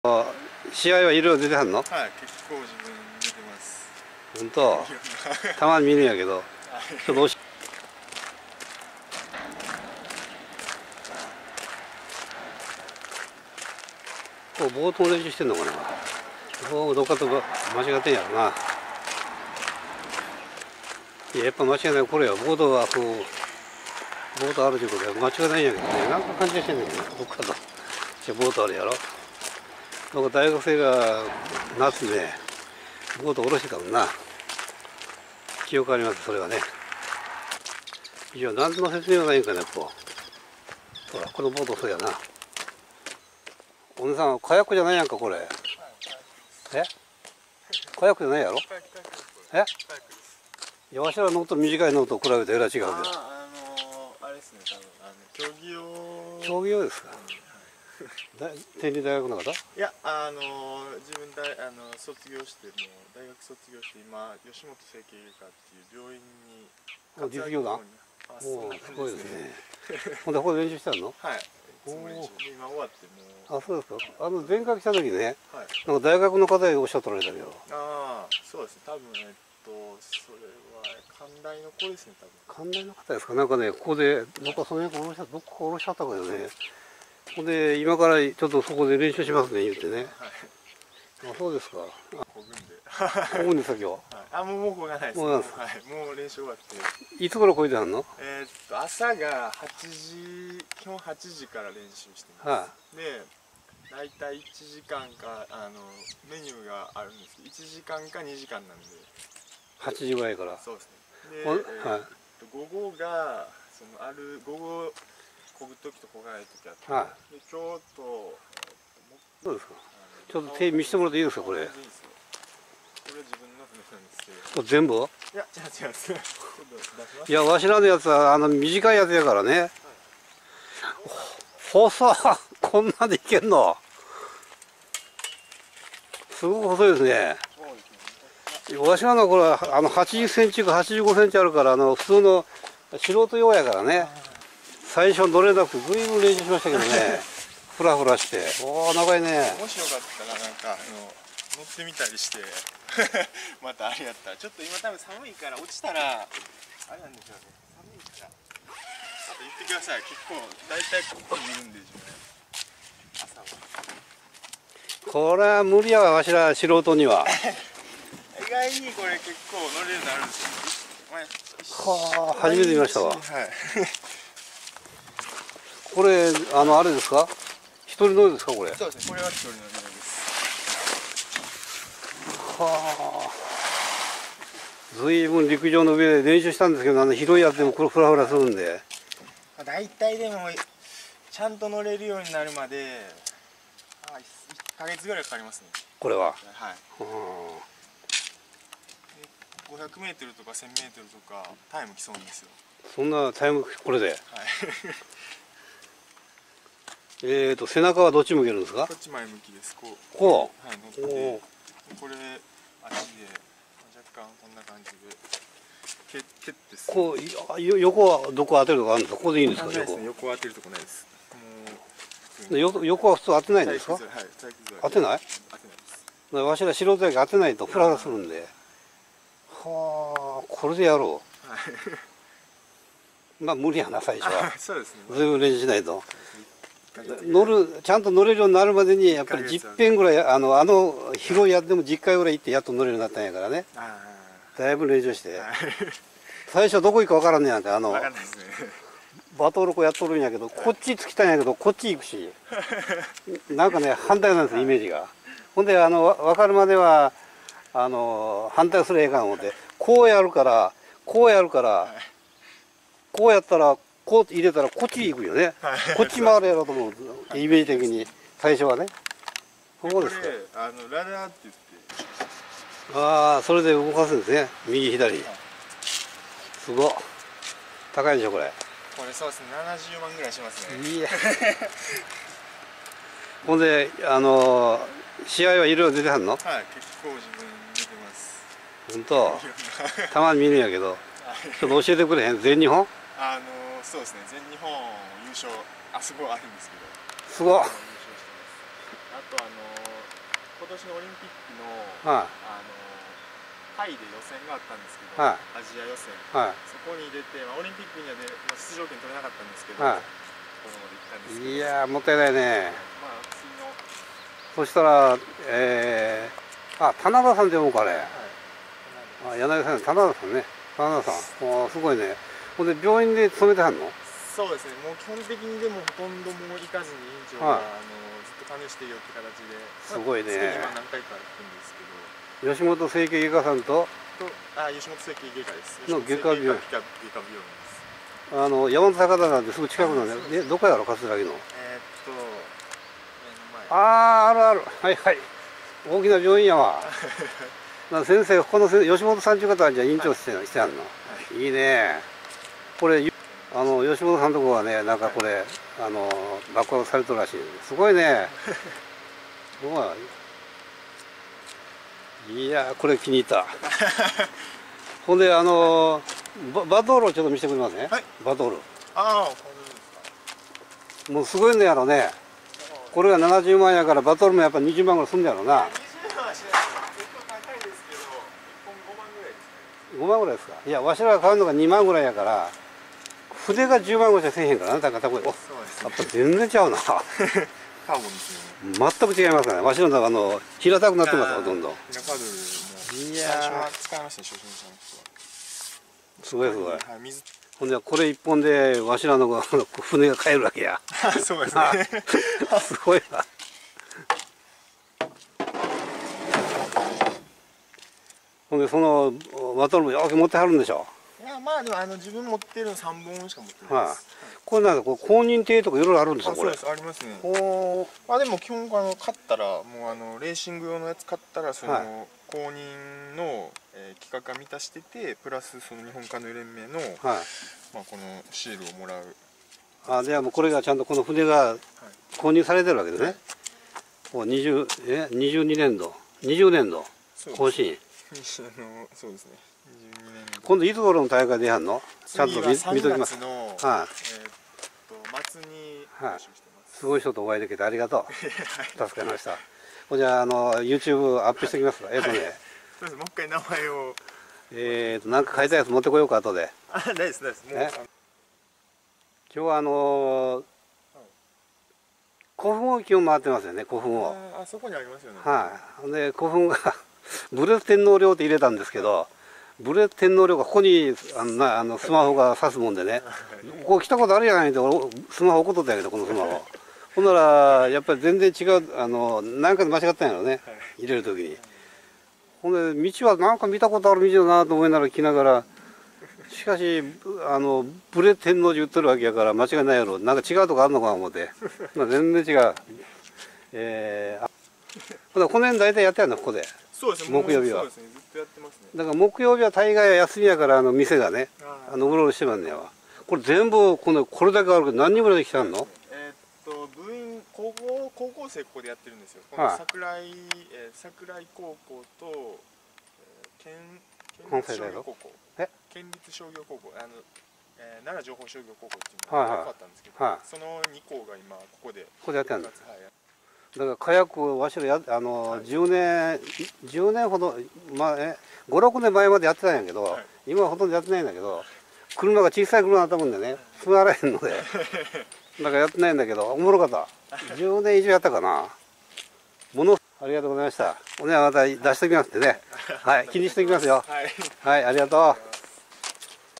さんに会う（男子カヤックシングル）来週木曜日に練習の撮影予定。（2021.11.11）（吉本整形外科・外科病院）人一倍の努力を立て、カヌー で世界の頂点をした。
人一倍の努力を重ねカヌーで世界の頂点を目指す。

小野が初の五輪代表　カヌー・スプリント
人一倍の努力を重ね、カヌーで世界の頂点を目指す。
棚田大志さん
K-1、1000ｍで優勝

2021年05月06日
カヌー・スプリントの東京五輪アジア予選は６日、タイのパタヤで行われ、女子カヤックシングル２００メートルの小野祐佳（秋田県スポーツ協会）が４０秒２５８で２位に入って五輪出場枠を獲得し、初の五輪代表に決まった。

男子カヤックシングル１０００メートルの棚田大志（吉本整形外科・外科病院）は３位、同２００メートルの小松正治（愛媛県競技力向上対策本部）は７位で出場枠を逃した。